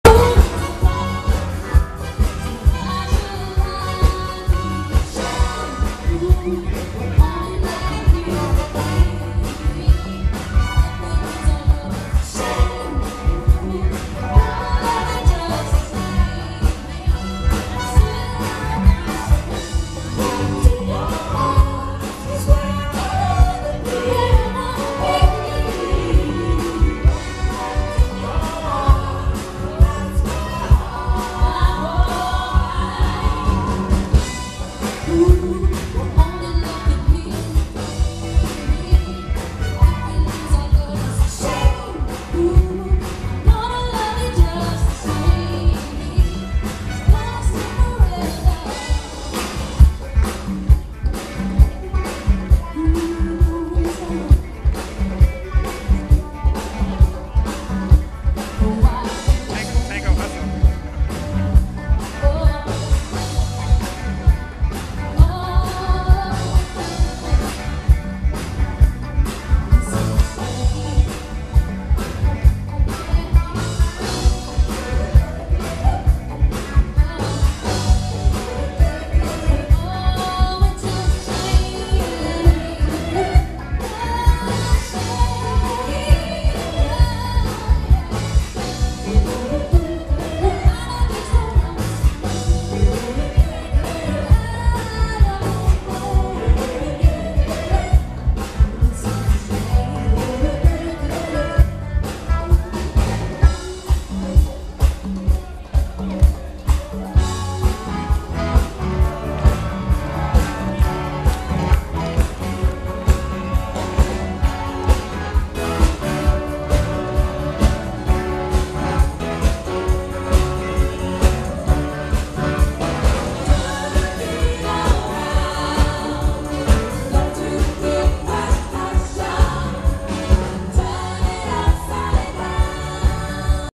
Hustle dancers MSNS
Dancing the hustle, Loser's Lounge: Ladies' Night, Midsummer Night Swing, Lincoln Center, NYC